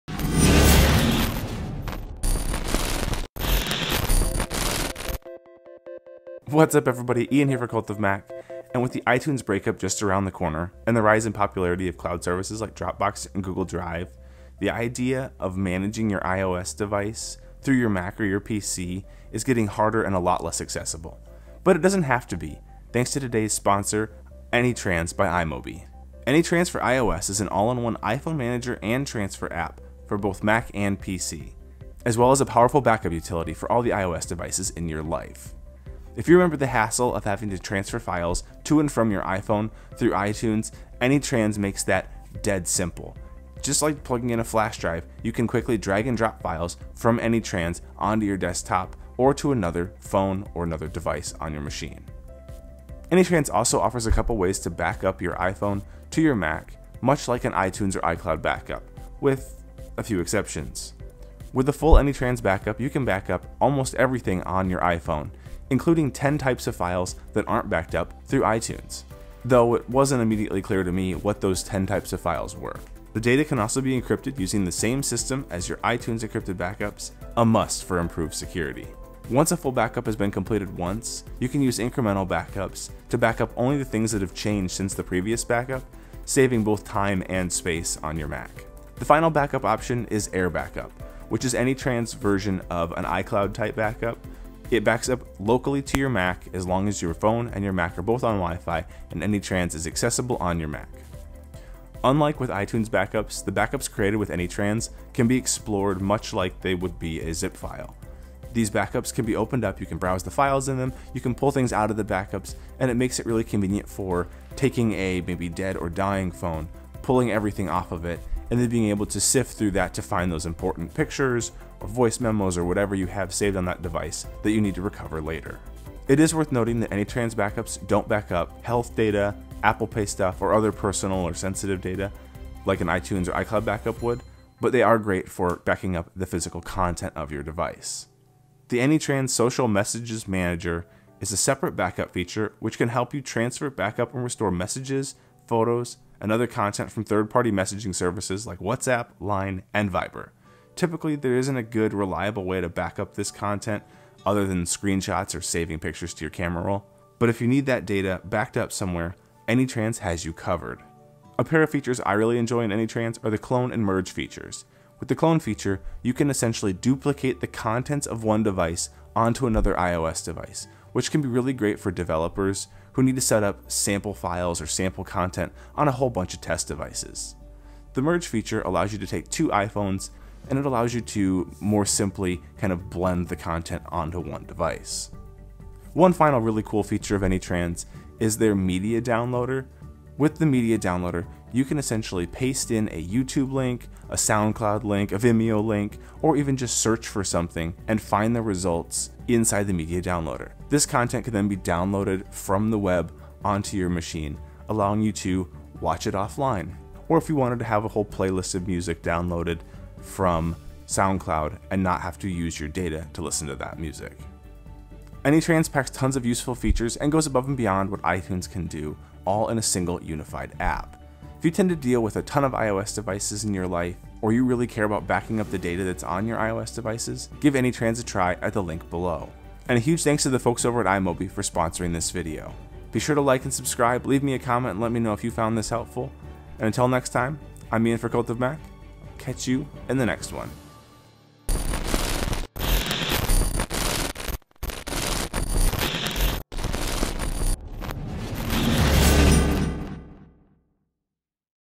AnyTrans : A Better Way to Manage Your iPhone's Files
iTunes is going away and everything is moving to the cloud - but that doesn't mean you have to give up backing up your iPhone or iPad locally.

AnyTrans is an iPhone manager from iMobie, which works on both Mac and PC.

As an one-stop iPhone manager, it can help users transfer, sync, back up, and manage iPhone data and files in the easiest way.
Better still, with it, you can custom iPhone ringtone, organize iPhone home screen, bulk download apps, transfer and backup WhatsApp messages, and more. It’s dead simple to use and an excellent tool for iPhone owners everywhere.